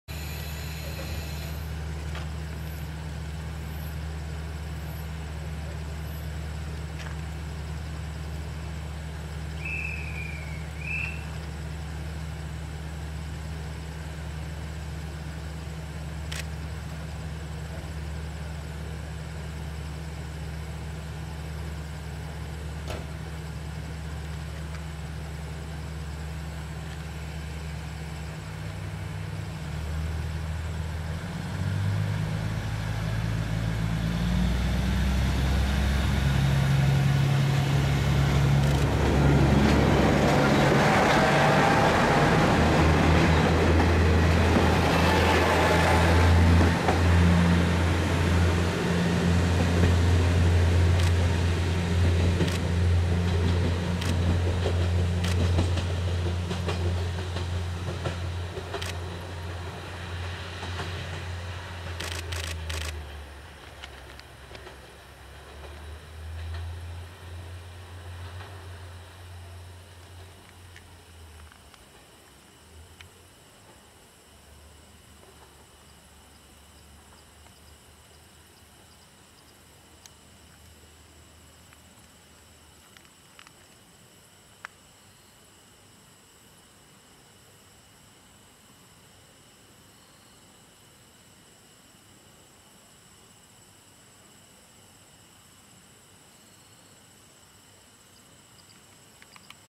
高山本線 杉原
最終列車が発車。テールランプが闇に消える。